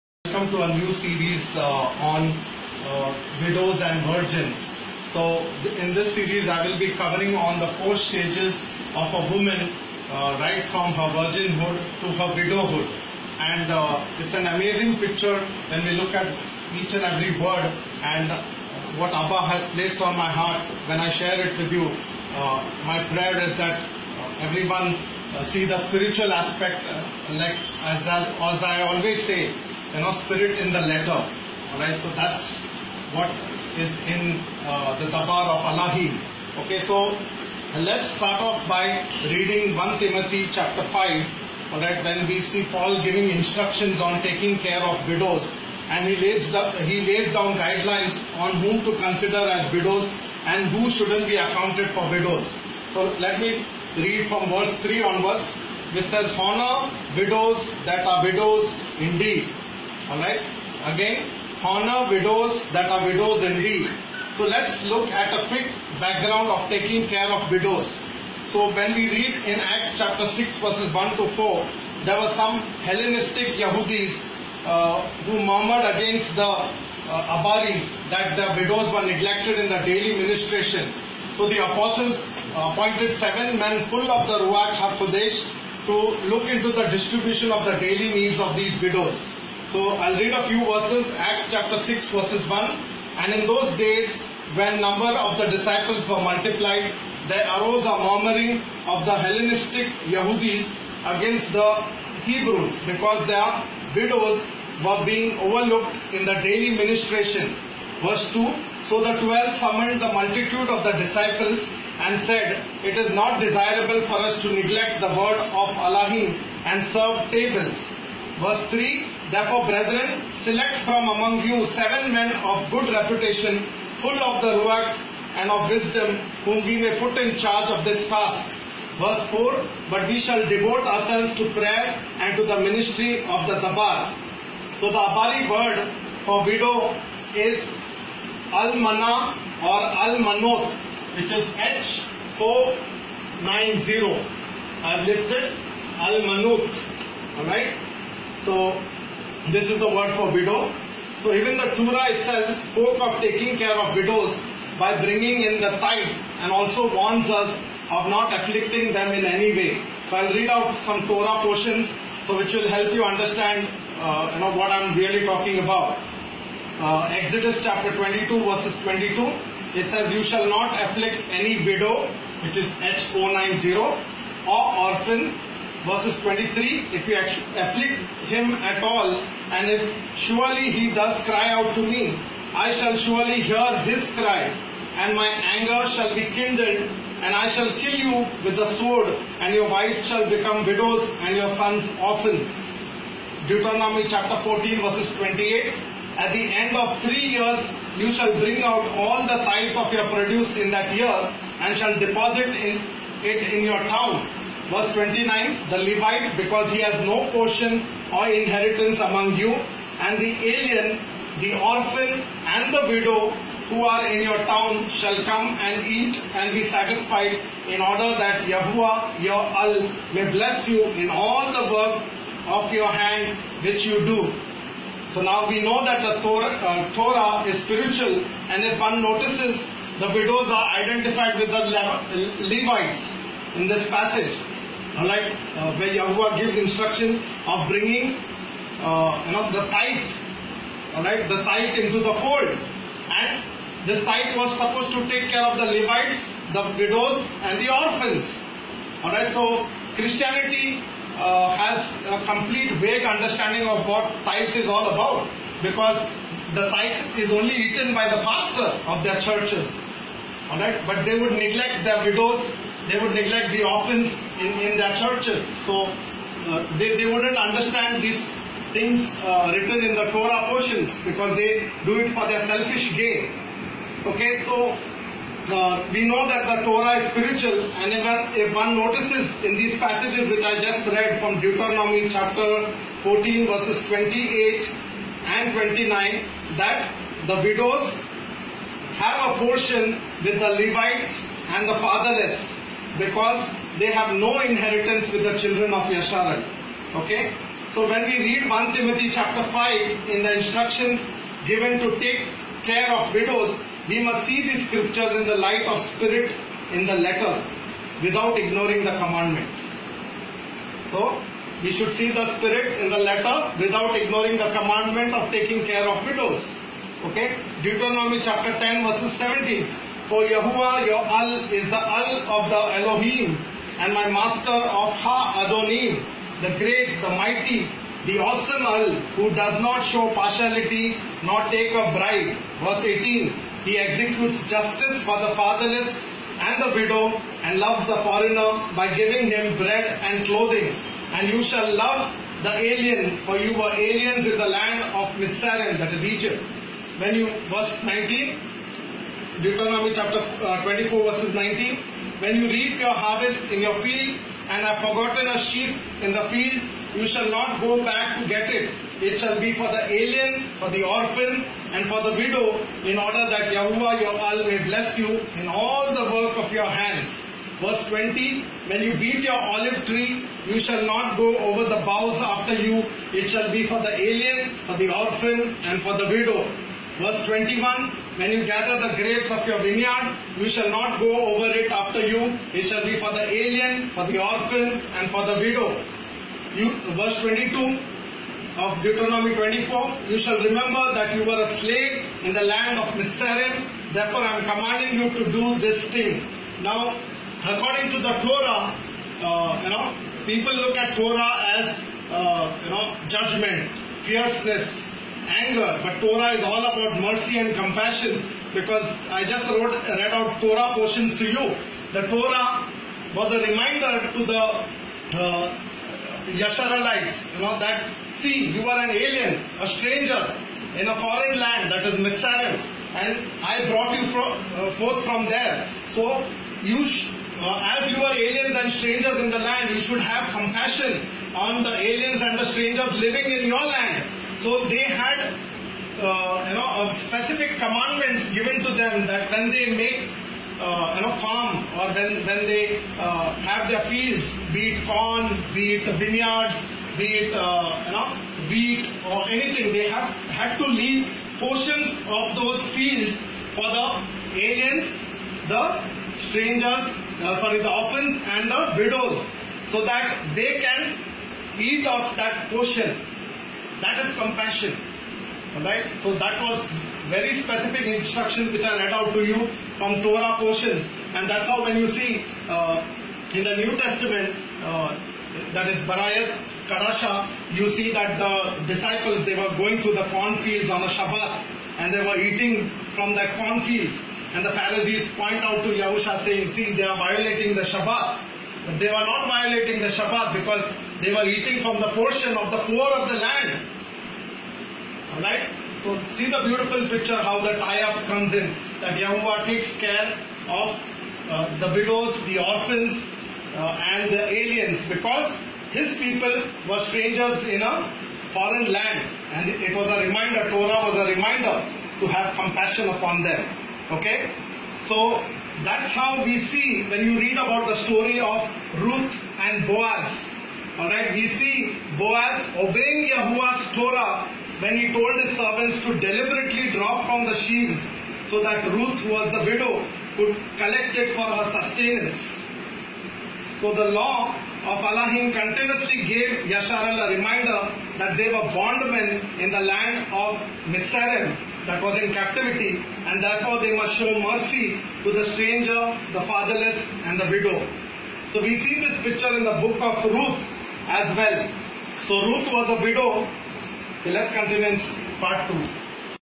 Widows & Virgins Part 1
These video series show the woman Yarashalam  four stages and how she finds her fulfillment and completeness in Yahusha ha Mashiyach.